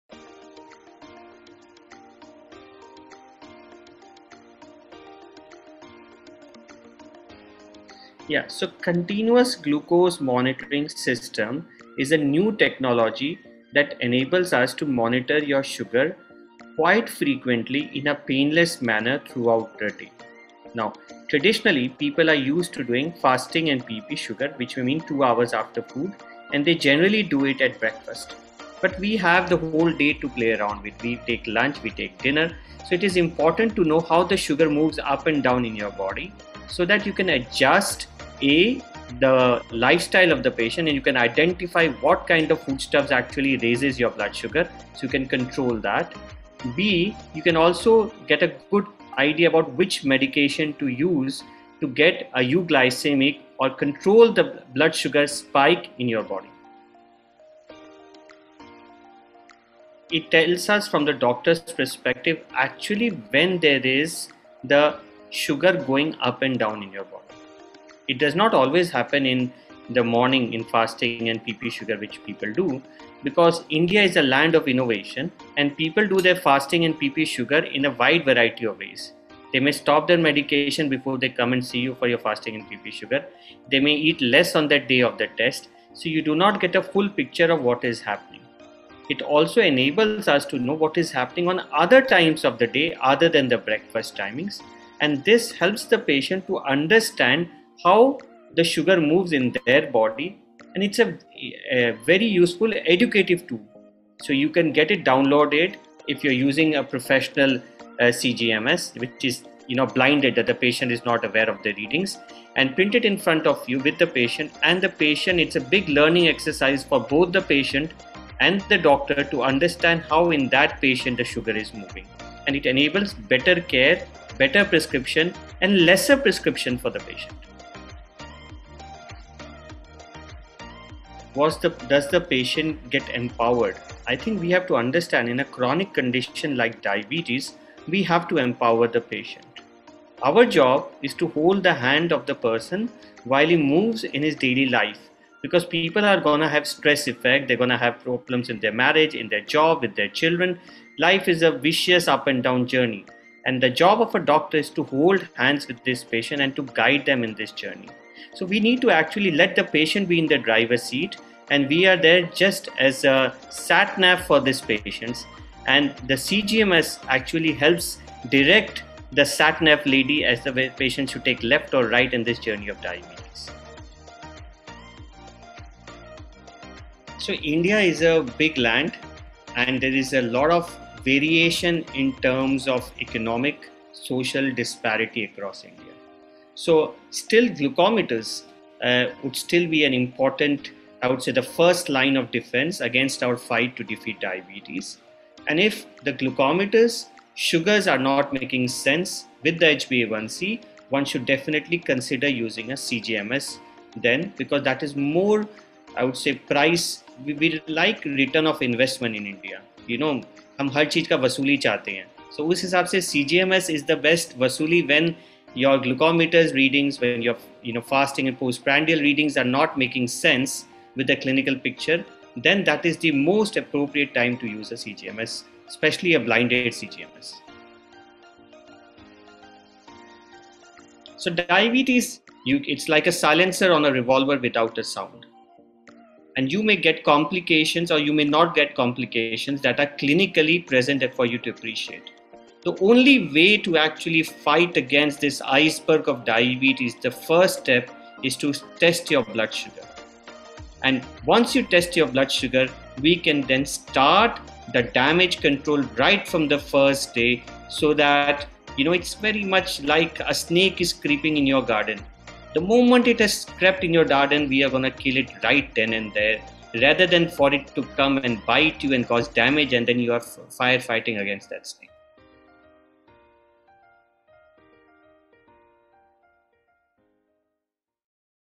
What is Continuous Glucose Monitoring (CGM) and How To Use It?
Dr Mithun Bhartia is a Consultant Endocrinologist and RSSDI National Technology Task Force Member. He recently engaged in an insightful conversation with us about Continuous Glucose Monitoring.

In this video, he talks about:
• What is Continuous Glucose Monitoring?
• How is it helpful?
• When should you use CGM?
• How it Empowers Patients and more..

Watch and learn here. ✅